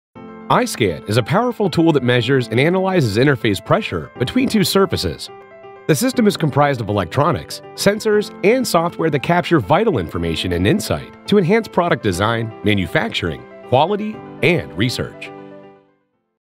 I-Scan: The Configurable Pressure Mapping Toolkit
Here's a brief glimpse of how I-Scan pressure mapping systems can be used to capture real-time interface pressure data between two surfaces. Learn more about how I-Scan can help provide insight into your R&D process